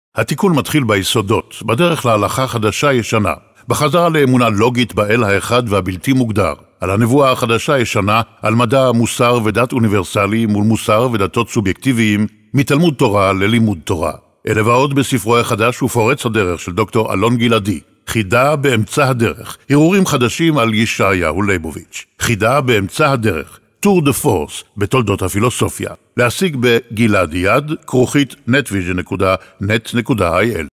חידה באמצע הדרך - אל האמונה הישראלית, אל הדת הישראלית, אל התורה הישראלית, ואל הנבואה הישראלית.
"חידה באמצע הדרך – הרהורים חדשים על ישעיהו ליבוביץ" -
התיקון מתחיל ביסודות.
בדרך להלכה חדשה-ישנה –
בחזרה לאמונה לוגית באל האחד והבלתי מוגדר,
על הנבואה החדשה-ישנה,
על מדע, מוסר ודת אוניברסליים מול מוסר ודתות סובייקטיביים,
מתלמוד תורה ללימוד תורה.
אלה ועוד בספרו החדש ופורץ הדרך של דר' אלון גלעדי –
"חידה באמצע הדרך" – "tour de force בתולדות הפילוסופיה".